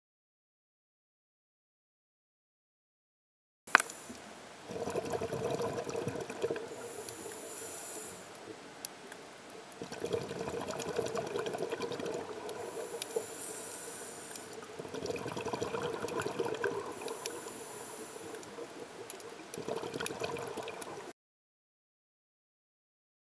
Seadragon under siege
Some kinda cowfish wanting to have some seadragon eggs for breakfast under flinders pier.